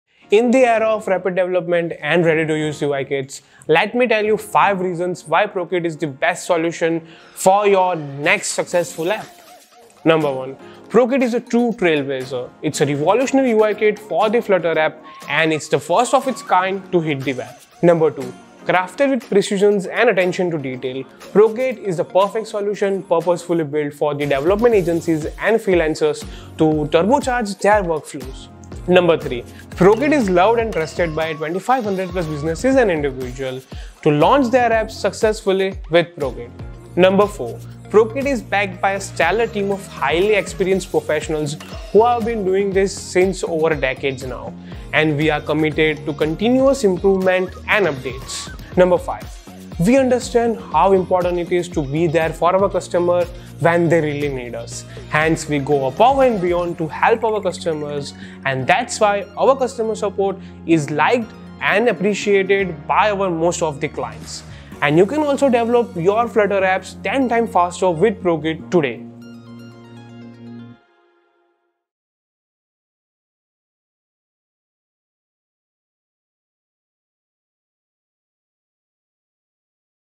What Makes ProKit Different? | Iqonic Design
Discover why Prokit - Biggest Flutter UI Kit" stands out from the competition with this informative video! Get an inside look at how Prokit has revolutionized the industry with features such as its easy-to-use interface, advanced customization options, and powerful analytics.

Hear from experts about their experiences using Prokit - Flutter UI kits, and find out exactly what makes it unique. See why Prokit is a top choice for businesses of all sizes and learn how it can help you stay ahead of the competition.